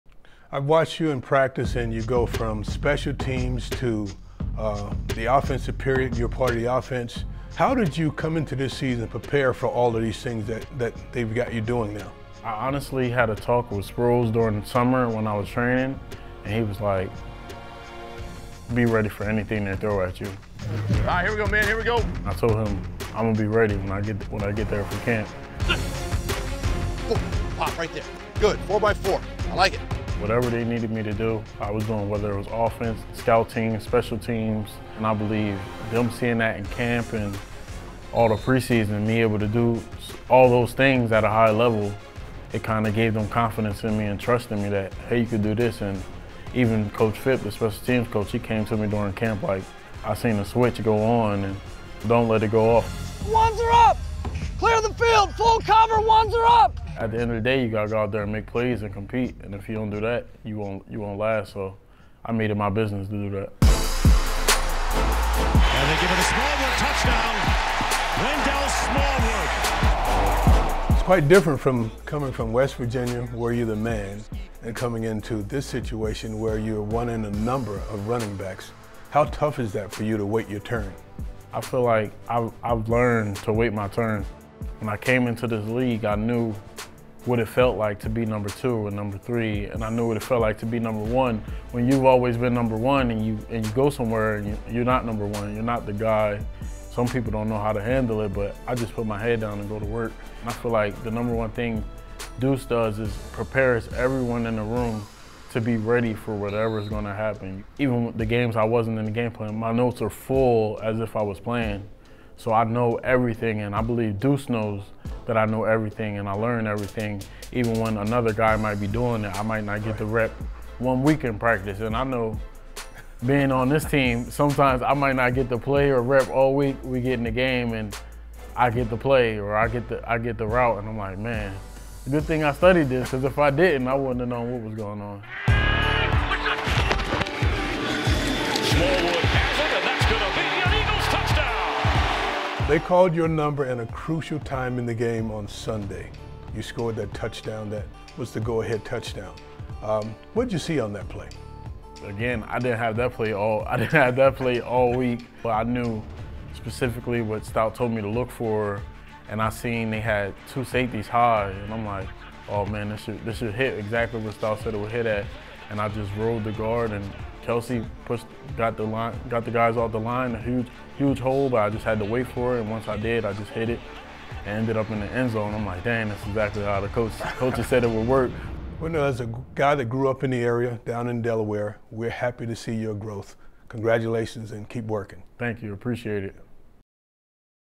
RB Wendell Smallwood Discusses His Evolving Role On The Team & More | Eagles One-On-One
Philadelphia Eagles running back Wendell Smallwood sits down with Mike Quick to discuss his progress this season and his touchdown play from last week's victory over the Indianapolis Colts.